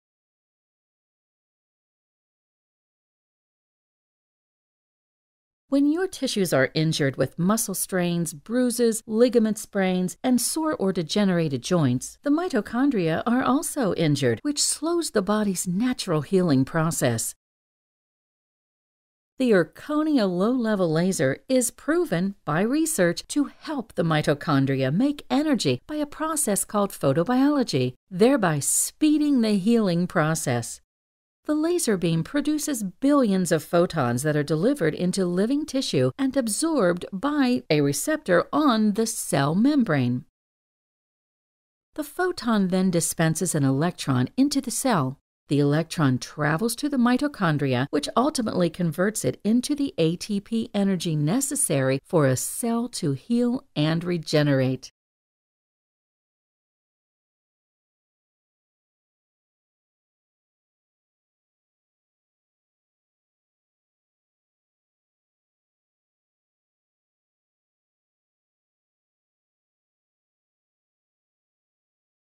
3D Patient Education: Low Level Laser Therapy for Neck and Shoulder Pain (Quick Version)
3D Patient Education prepared for Roger Tischner, D.C.
Like this video?  Shop our Customized Patient Education Videos and LLLT Waiting Room Videos